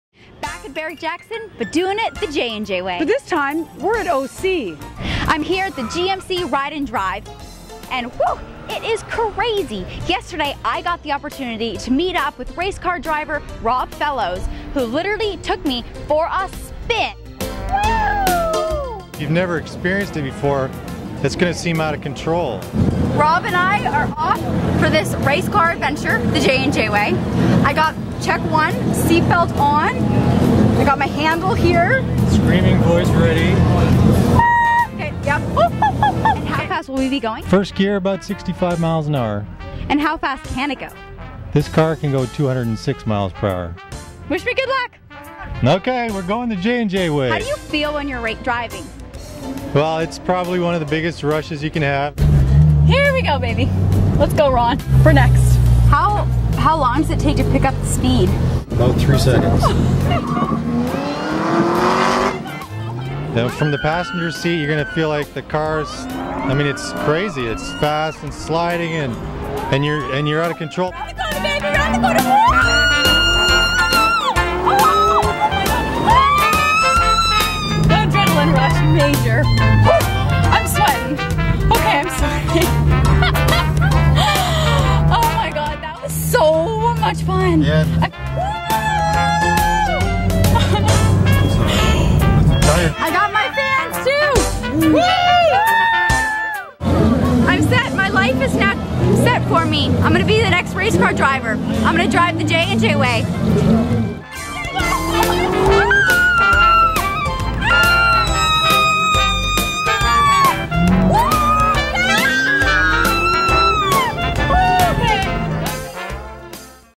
Barrett Jackson OC GMC Hot Lap
J&J did the Ride N' Drive GMC Hot Lap at the Barrett-Jackson Classic Car Auction in O.C.